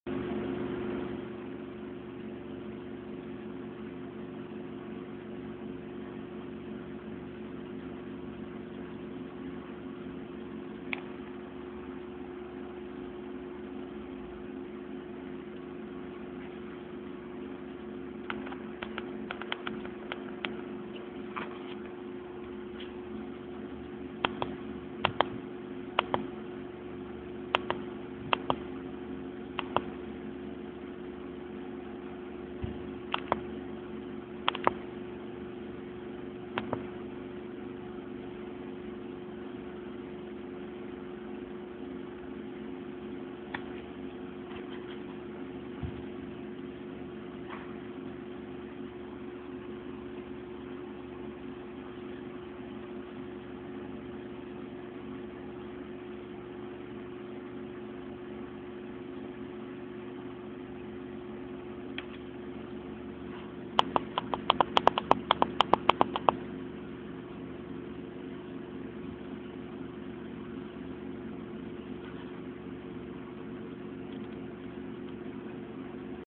triple arranque mandriva vista y xp
triple arranque en una pc entre Madriva, vista y xp iniciando con grub